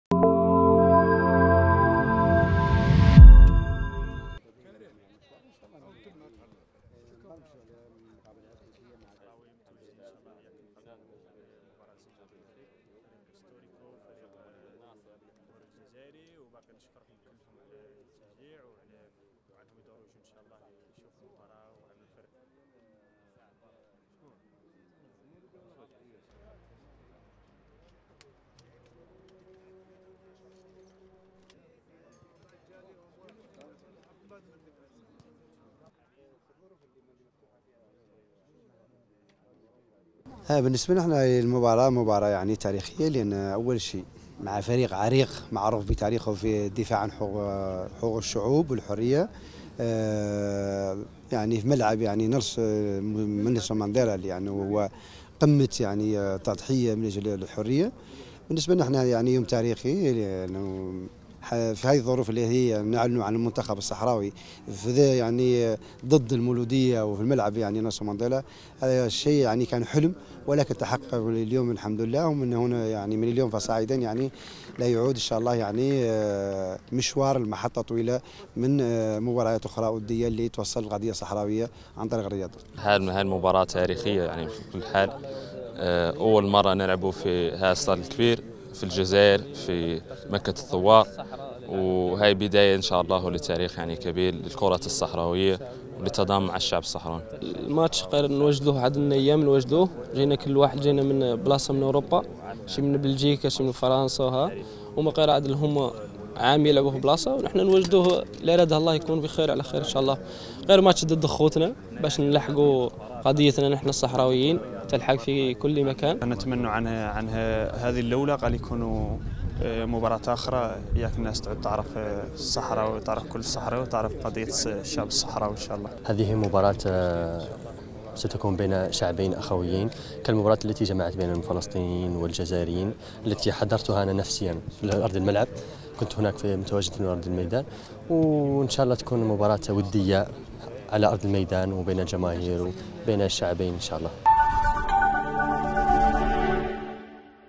منتخب الصحراء الغربية يجري حصة تدريبية استعدادا لمواجهة مولودية الجزائر
الجزائر(وأج)- اجرى منتخب الصحراء الغربية حصة تدريبية بملعب 5 جويلية، وذلك استعدادا لمواجهة مولودية الجزائر، المقررة
 بملعب نيلسون مانديلا ببراقي (الجزائر العاصمة) .